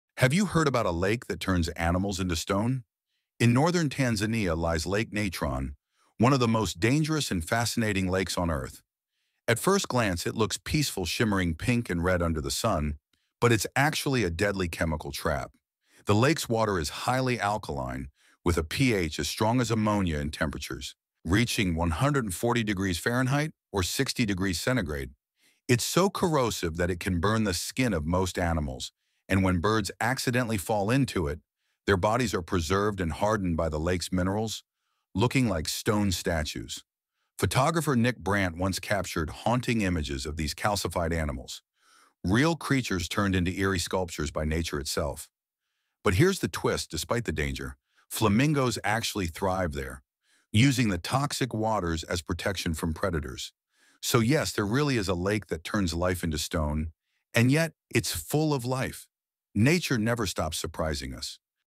The Lake That Turns Animals Into Stone!
Discover the mysterious Lake Natron in Tanzania — a deadly alkaline lake that preserves animals like statues. A real, verified natural phenomenon that proves nature can be both beautiful and terrifying. 🪨🌋

✅ Verified Sources:

National Geographic (Lake Natron’s deadly chemistry)

BBC Earth (Flamingos and Lake Natron ecosystem)

Smithsonian Magazine (Nick Brandt photography and lake preservation effects)